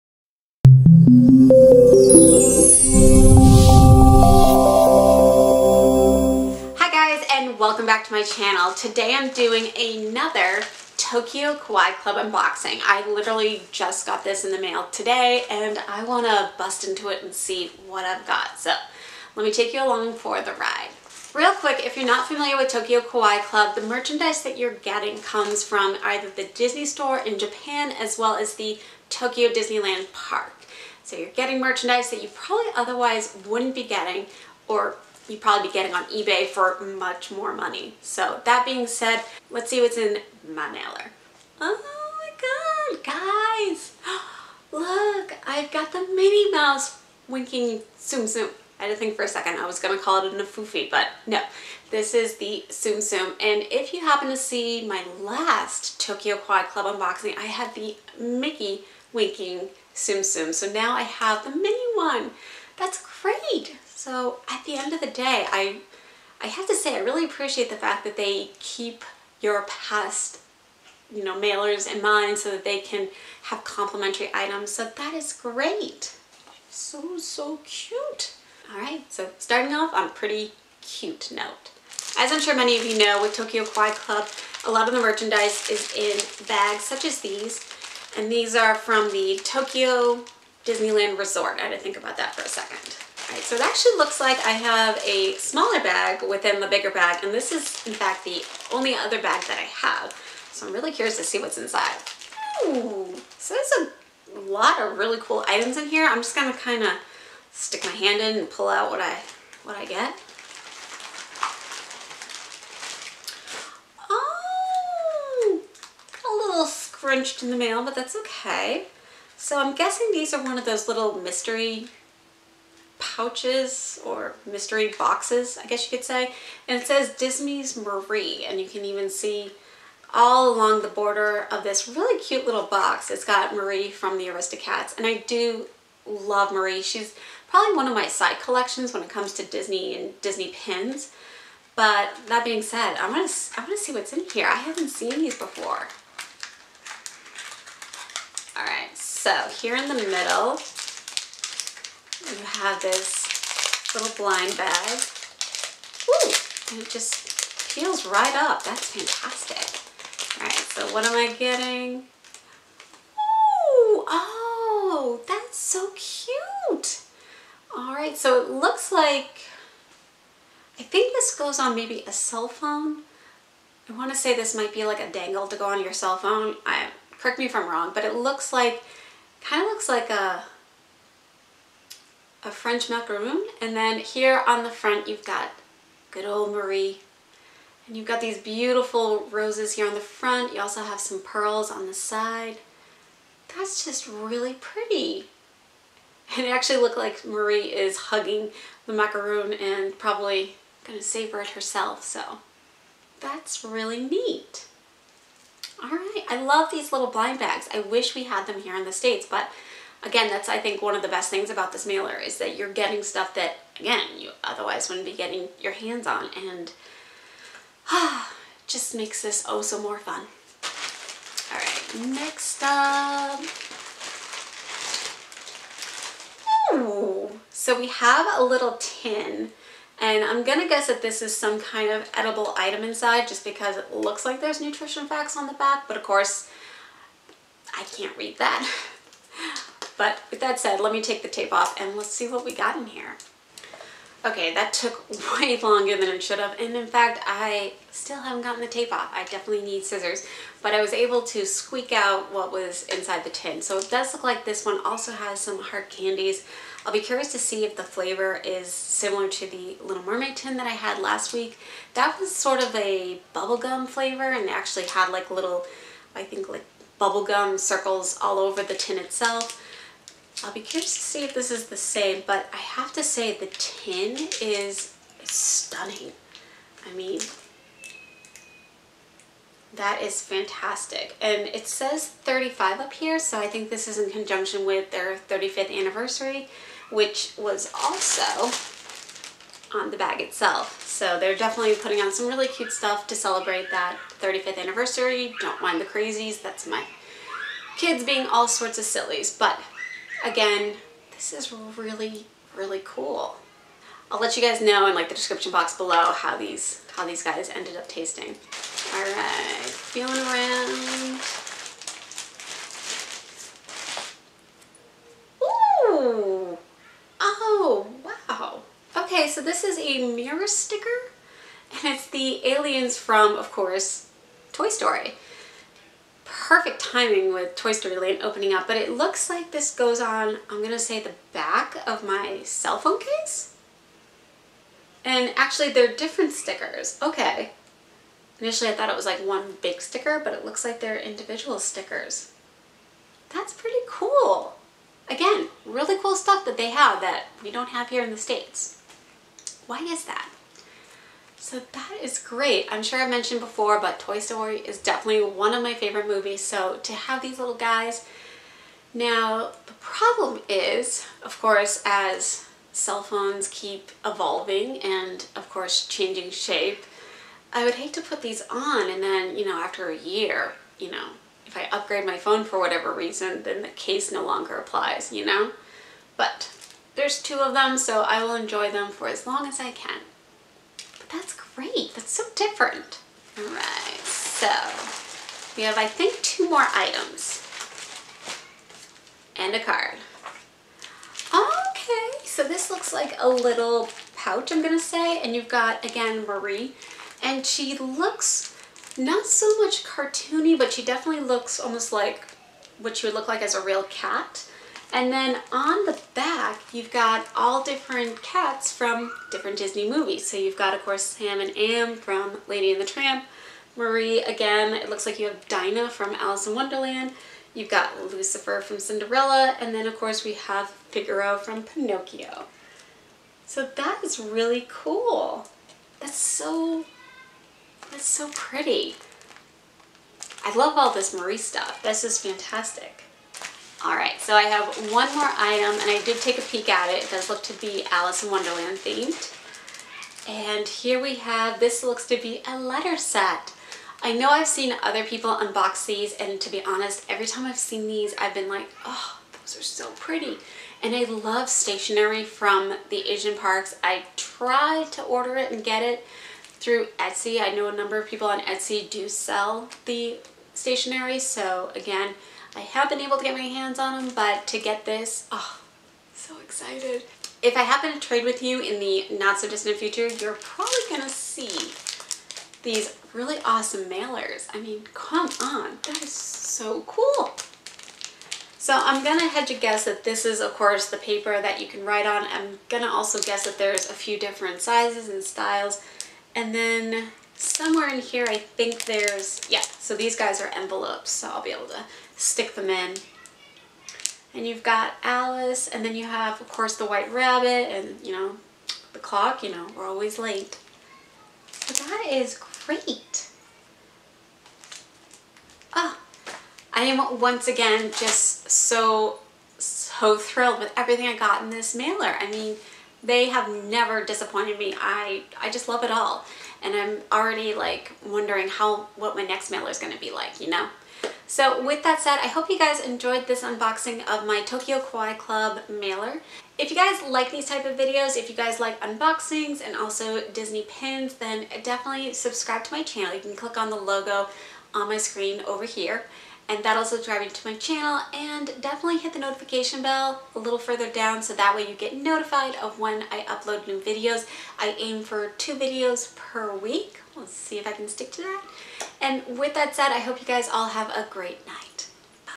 Disney Unboxing - Tokyo Kawaii Club - July Edition
Check out this month's Tokyo Kawaii Club unboxing.  This has become one of my favorite subscription boxes. The various items from the Disney Store in Japan and Tokyo Disneyland Resort make for one unique unboxing.

If you want to learn more about Tokyo Kawaii Club click the hyperlink below.

Facebook: Pins and Pixie Dust